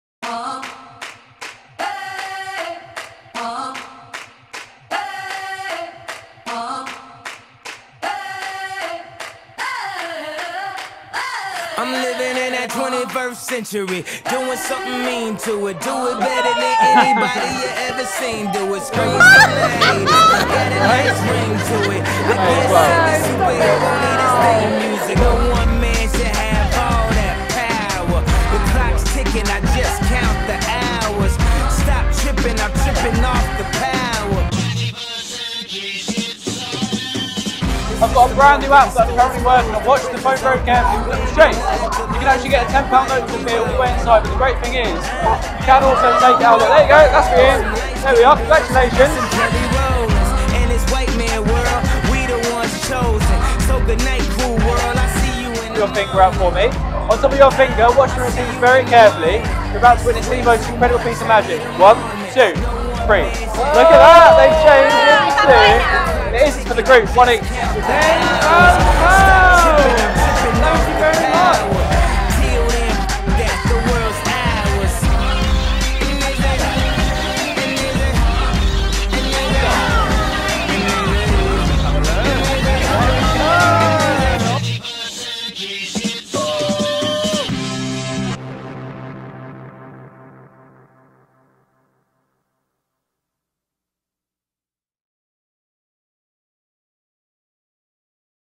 Dean Leavy: Magician Showreel 2025
Experience the magic of Dean Leavy in this 2025 showreel, featuring some of his best performances at weddings, corporate events, exhibitions/trade shows and more. and enquire today.